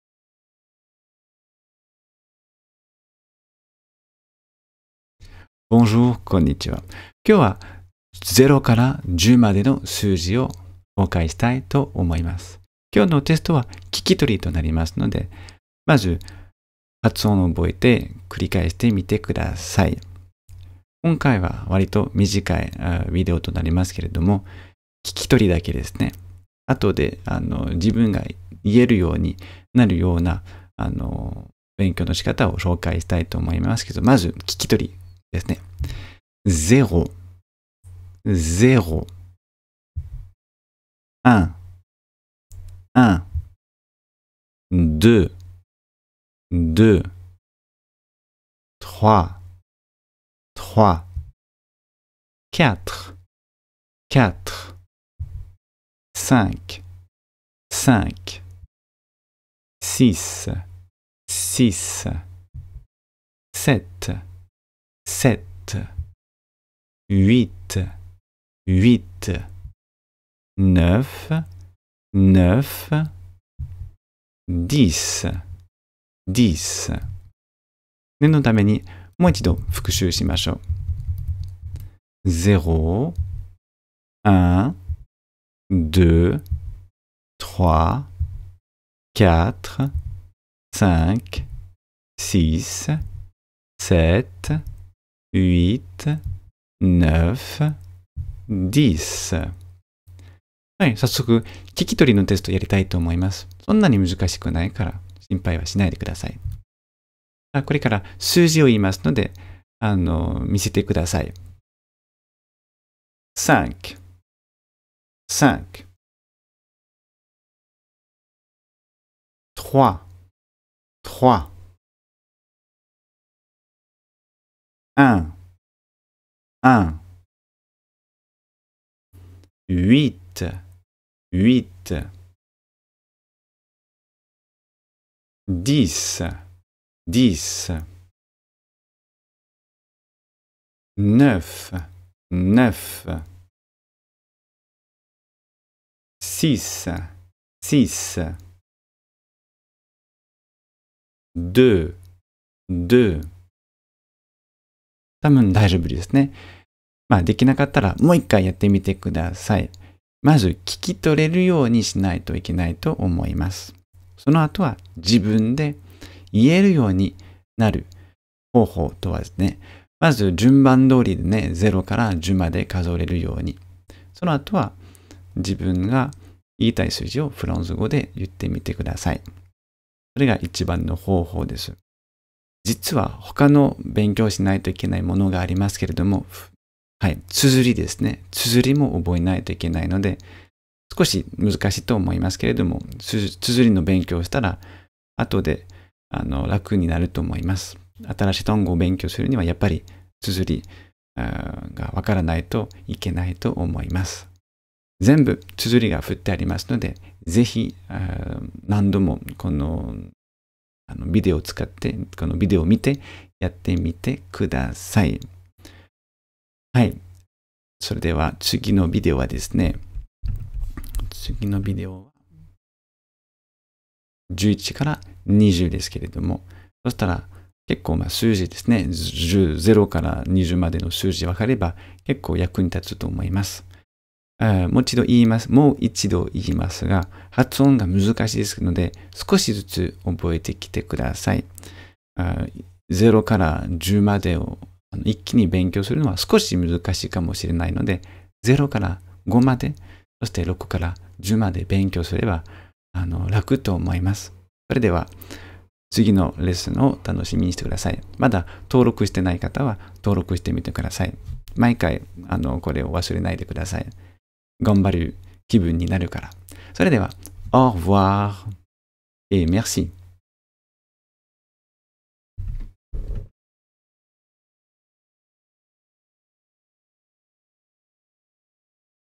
フランス語の数字 発音 0~10
フランソワ先生と一緒に、フランス語の数字「0～10」までを学びましょう。講師の口の開け方や動きをしっかり確認し、効果的にフランス語の数字をマスターししょう！